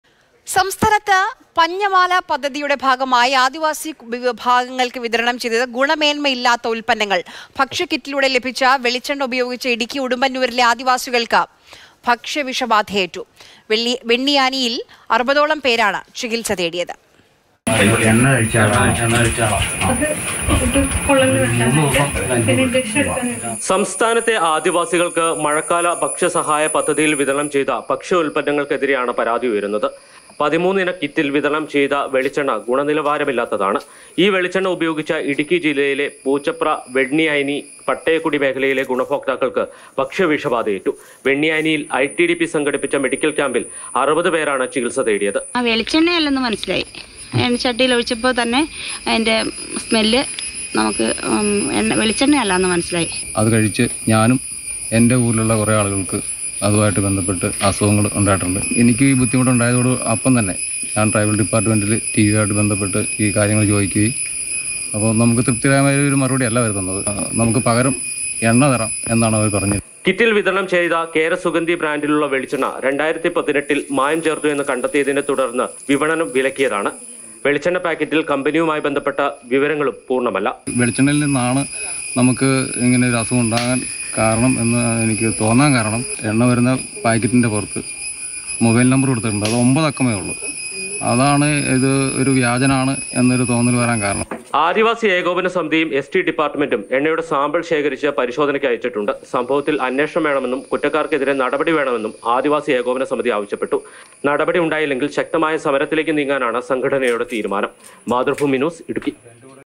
ഭക്ഷ്യസഹായ പദ്ധതിയിൽ മായം; ആദിവാസികൾക്ക് നൽകിയത് ഗുണമേന്മയില്ലാത്ത ഉത്പന്നങ്ങൾ
Happy viewing!